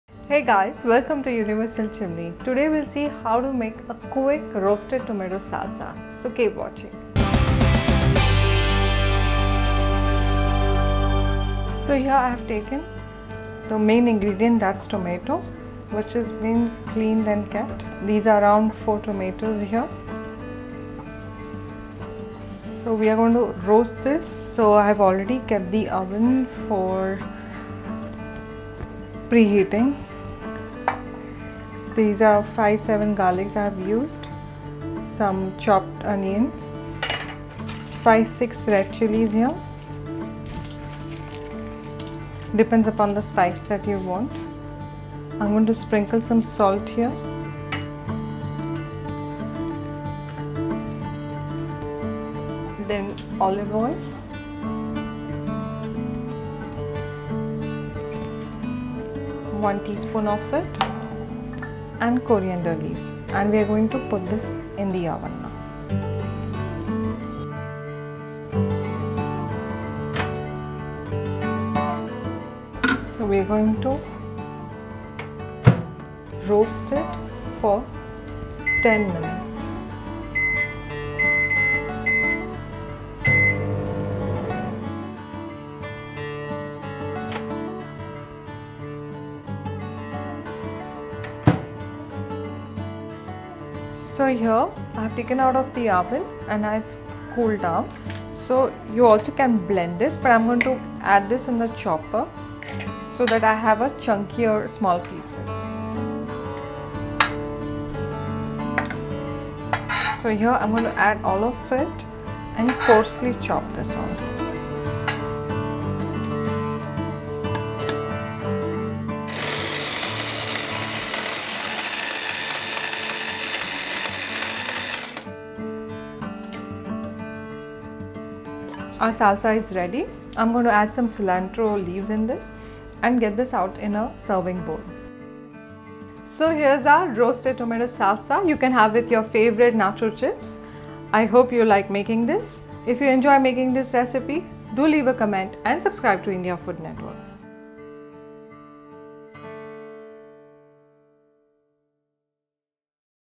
Roasted Tomato Salsa Sauce At Home By Smita | Universal Chimney
Roasted Tomato Salsa Sauce is the best tangy accompaniment for Nacho Chips. If you're looking for a simple and quick snack to offer your guests, this Salsa Sauce recipe teamed up with Nacho Chips, of course will be a complete hit! Watch this video as Smita helps you quickly learn how to make tomato salsa sauce at home!

Serves- 4-5

You will require the following ingredients to learn how to cook roasted tomato salsa sauce at home:

4-5 Tomatoes (roasted)

5-6 Dried Chillies (as per taste)

½ Red Onion

Cilantro

5-6  Garlic Cloves

This is awesome, you now know how to make salsa sauce at home! Try it out with some yummy home made nacho chips.